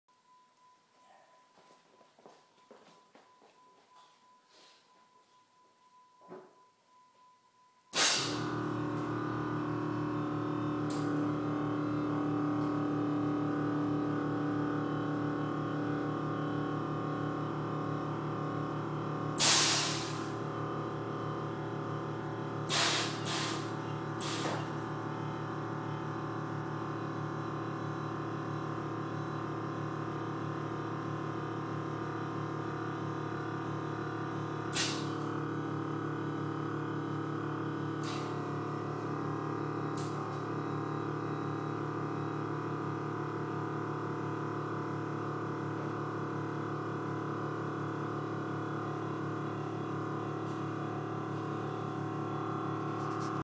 Universal air suspension testing
Playing with the system a little testing fill times and checking for leaks

Chrysler 300 air suspension